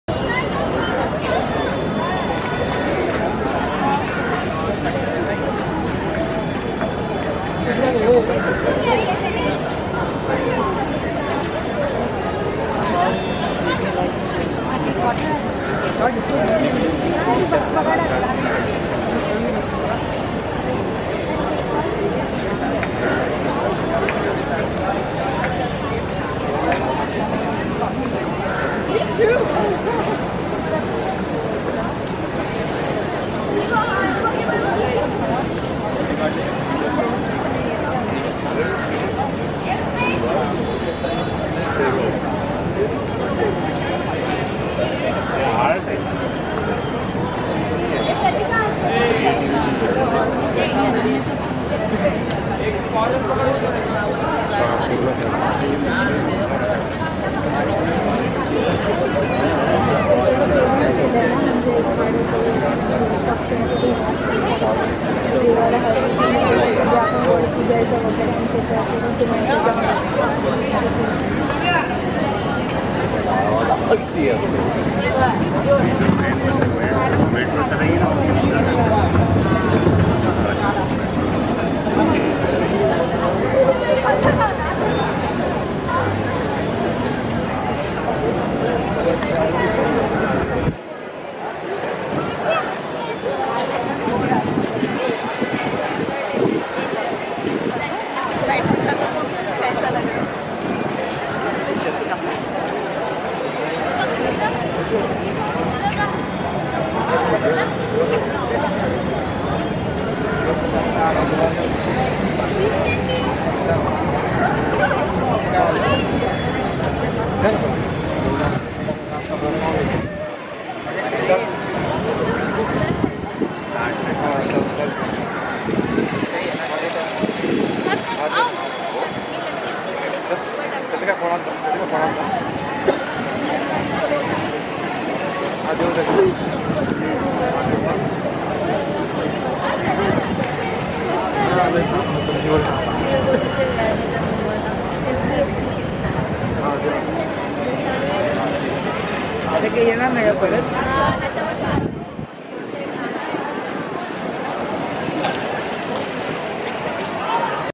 Concretization - Kala Ghoda 2018
Art beautifully portrayed “concretisation”. A half cut tree on which concretisation is shown which miniature skysraper turning the city into an urban heat island.